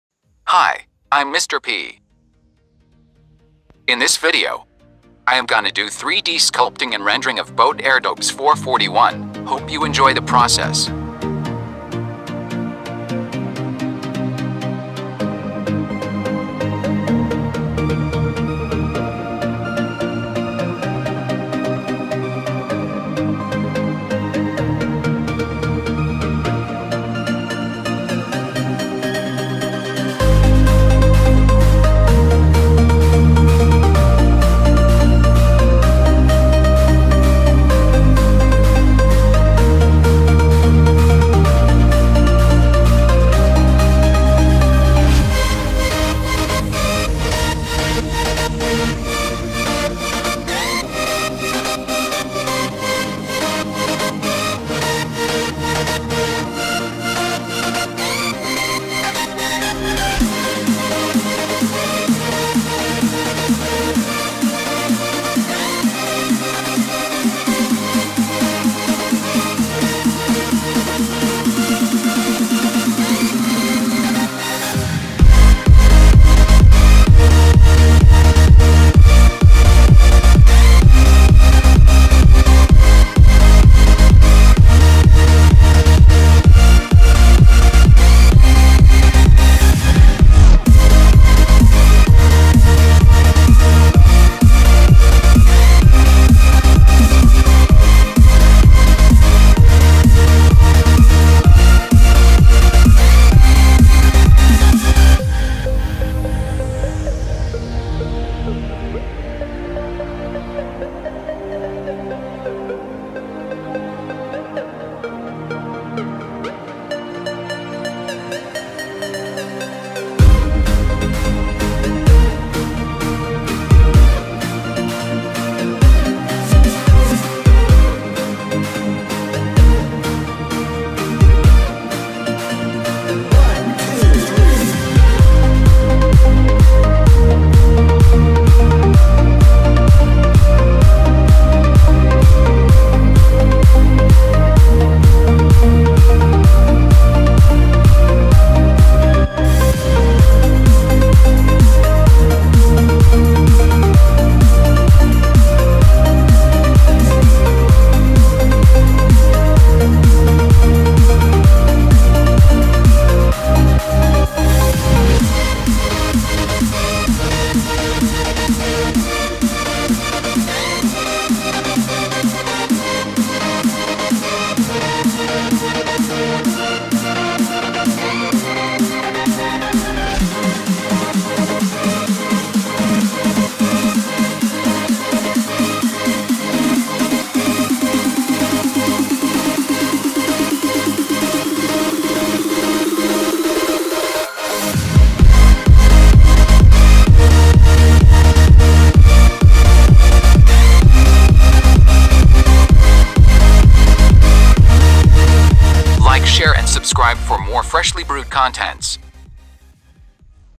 bOAt || Modelling & Rendering boat airdopes 441 in Zbrush || DIGITAL SCULPTING|| TIMELAPSE || Mr.P
I wanted to create a 3D model of the bOAt airdopes 441 as I bought them and the sound quality is awesome, hope you enjoy the video.
and please...

My Equipment:
- Photoshop CC
- Zbrush
- Huion Kamvas 16 Pro
- After Effects